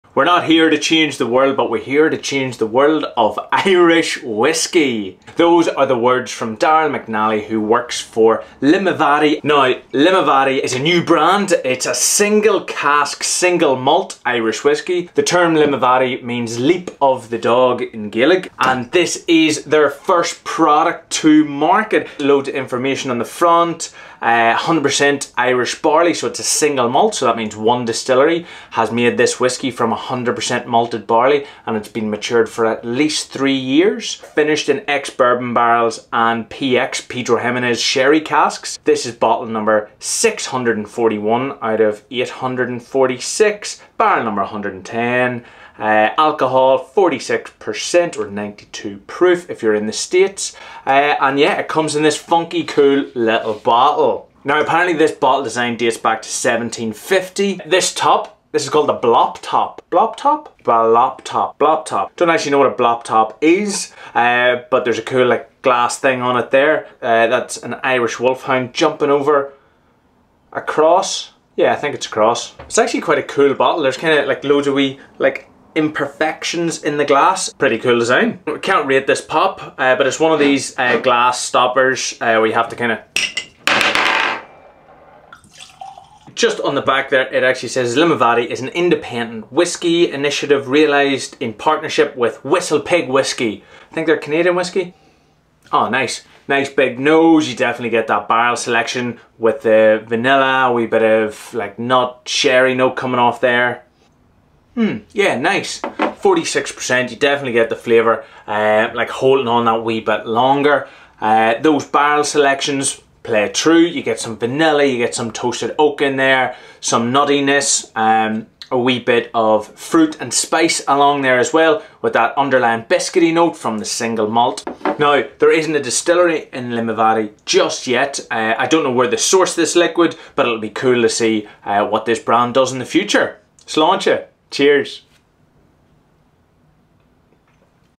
Limavady Single Malt | Irish Whiskey Review
Can you open a BLOPP TOP?!
Currently this is the only single barrel single malt Irish Whiskey on the market to date.
Distilled with 100% malted barley then triple distilled before being aged in ex-bourbon barrels, and finished in Pedro Ximenez sherry casks.